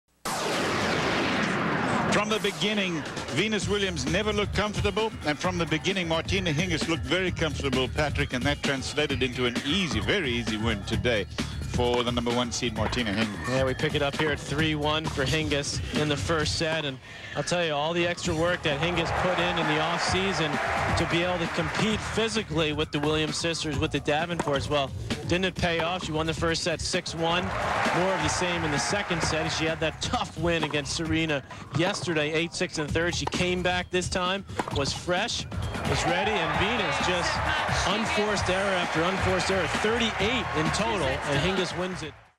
Hingis vs Venus highlights Australian Open 2001
Martina Hingis vs Venus Williams highlights semifinal Australian Open 2001